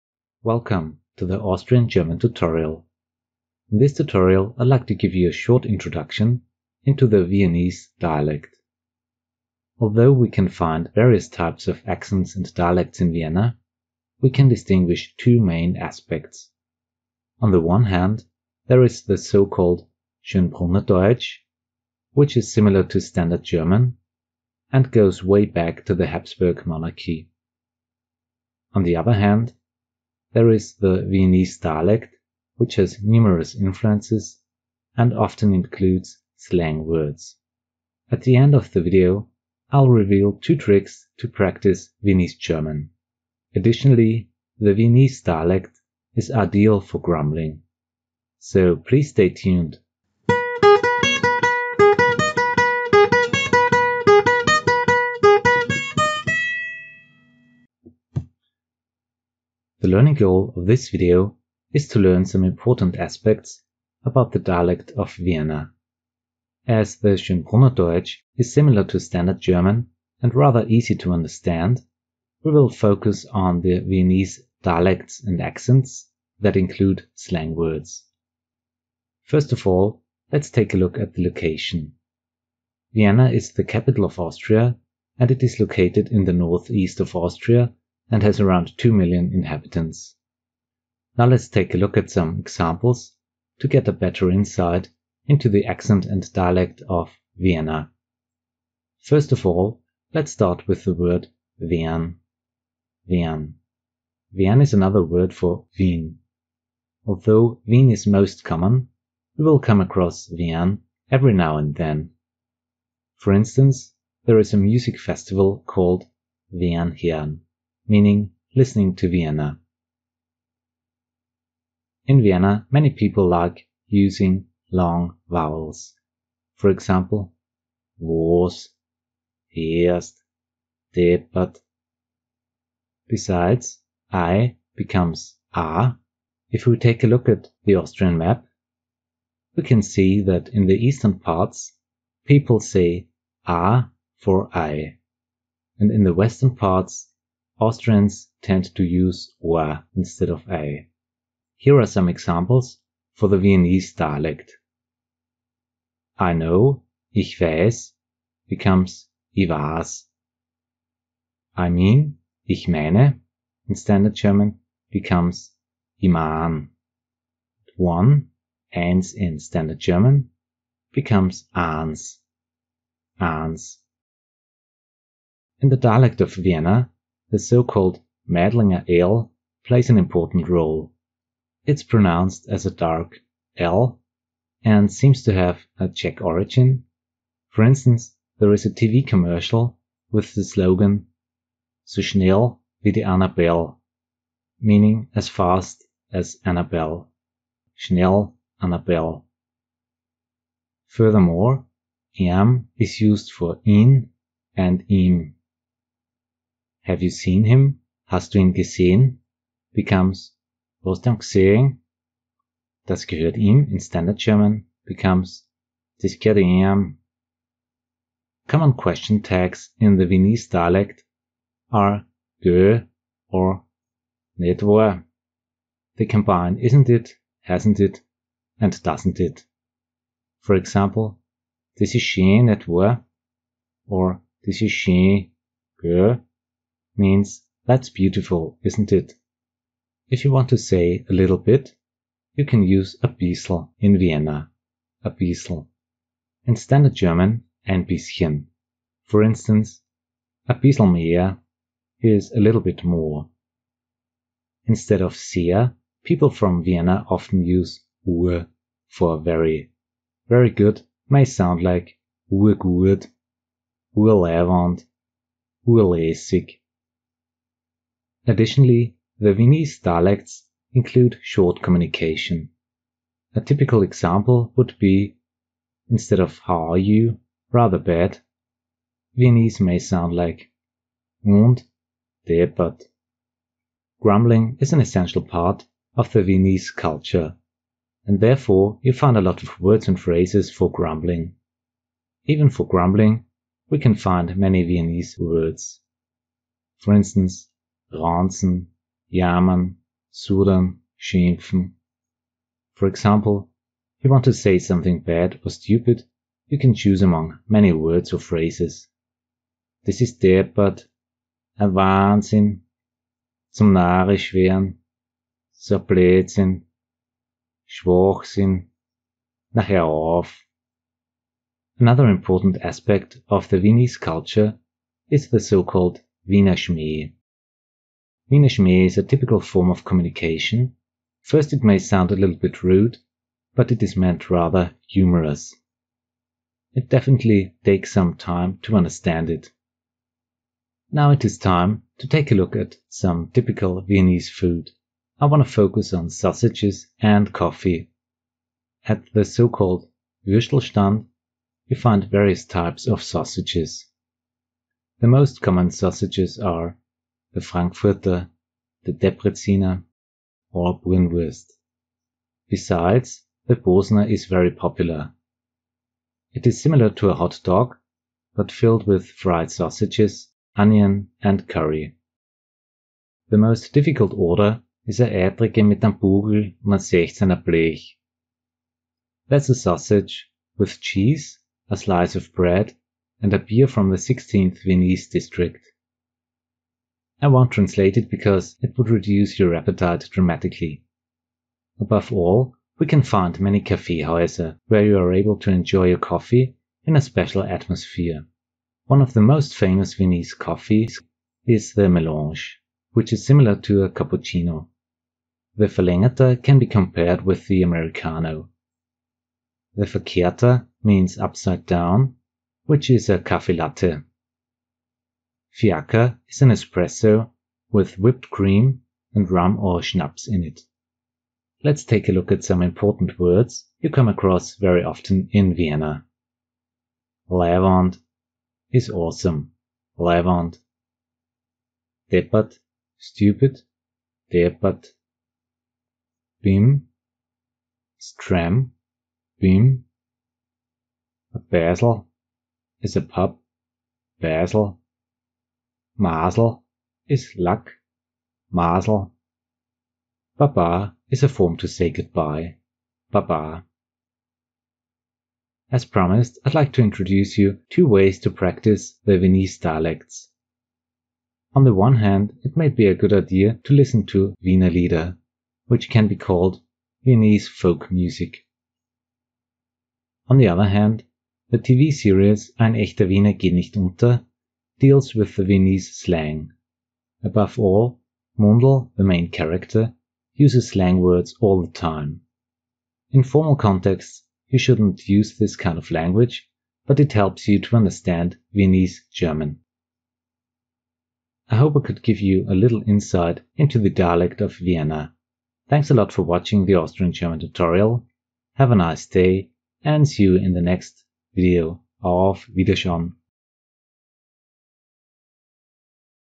Viennese Dialect - A Short Introduction
Welcome to the Austrian German Tutorial. In this Tutorial I'd like to give you a short introduction into the Viennese Dialect. Although we can find various types of accents and dialects in Vienna, we can distinguish two main aspects: On the one hand, there is the so called Schönbrunner Deutsch, which is similar to Standard German and goes way back to the Habsburg Monarchry. On the other hand, there is the Viennese Dialect, which has numerous influences and often includes slang words. At the end of the video I reveal two tricks how to practise Viennese German.

The learning goal of this video is to learn some important aspects about the dialect of Vienna. First of all, let's take a look at the location. Vienna is the capital of Austria and it is located in the north-east of Austria and has around two million inhabitants.

Now, let's take a look at some examples to get a better insight into the accent and dialect of Vienna.
First of all, let's start with the word WEAN: Wean is another word for Wien. Although Wien is most common, you will come across Wean every now and then. For instance, there is a music festival called, Wean hean, meaning listing to Vienna.
In Vienna many people like using long vowels.